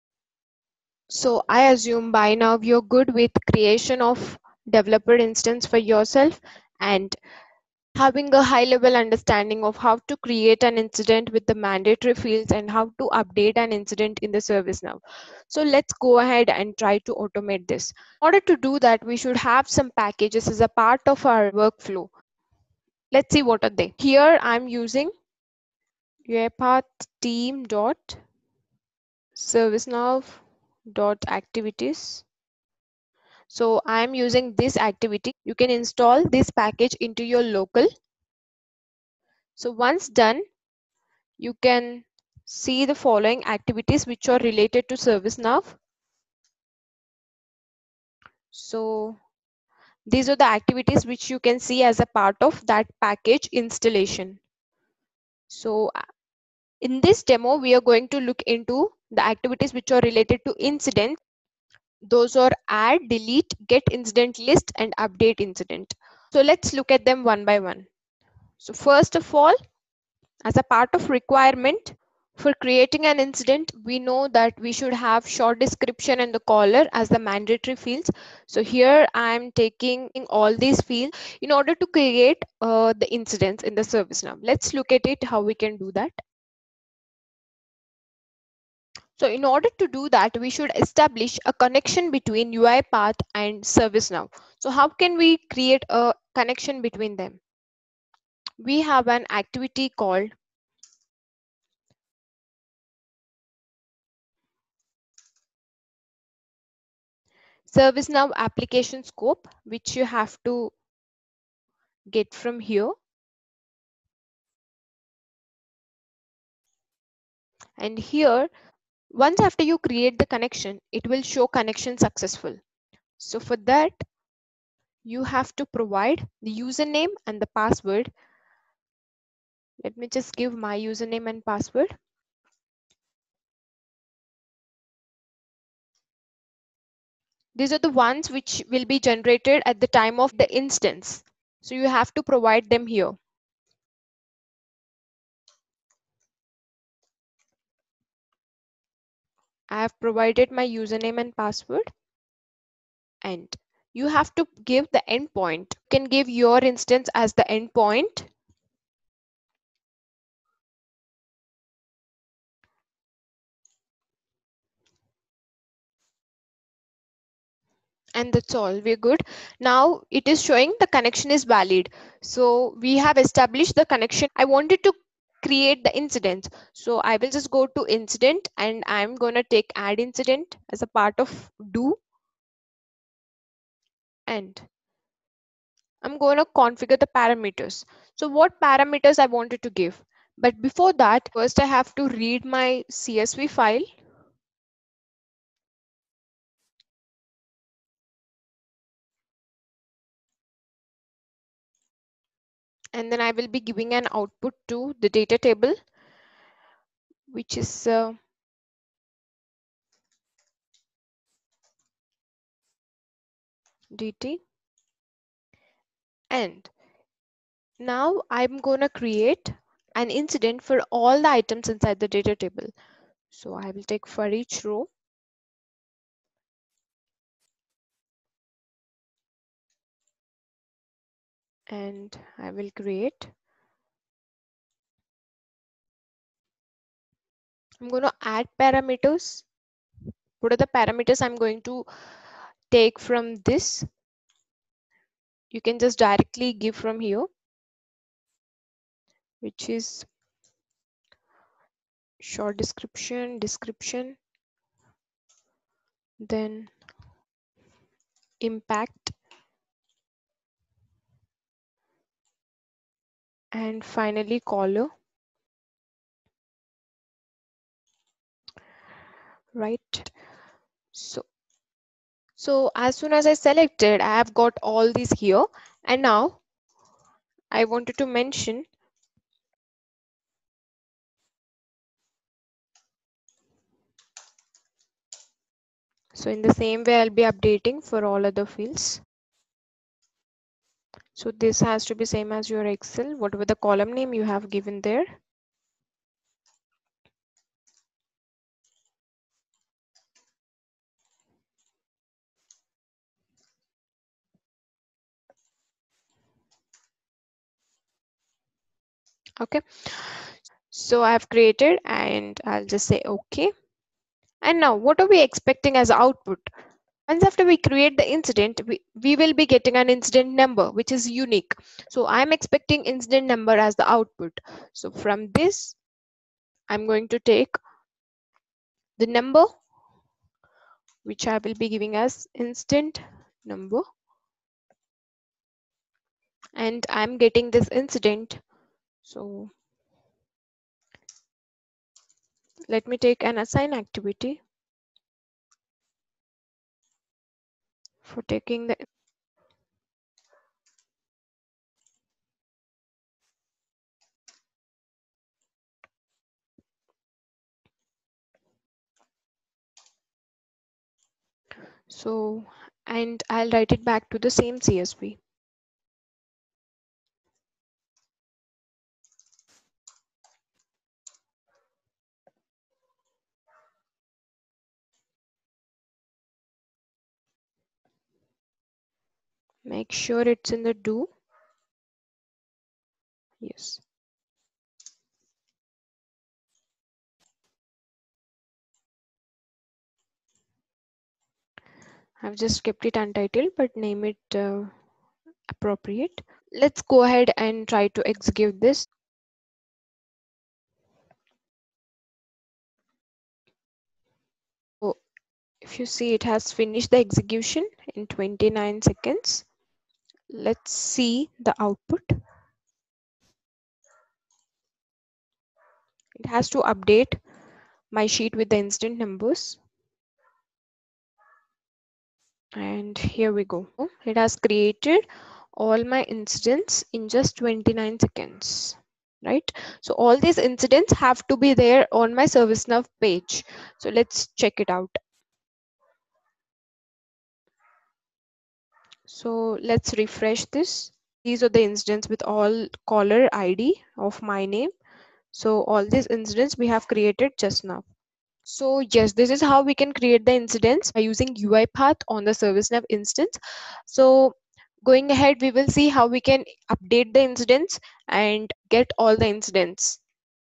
Servicenow with UiPath - How to automate creating incidents in uipath - Part3
As a part of this playlist you will understand how we can integrate UiPath with ServiceNow
By end of this series you will able to
1.Create developer instance to yourself on servicenow
2.Automate Creation of  Incidents on Service now platform
3.Automate get all  Incidents list from Service now platform
4.Update all incidents through automation.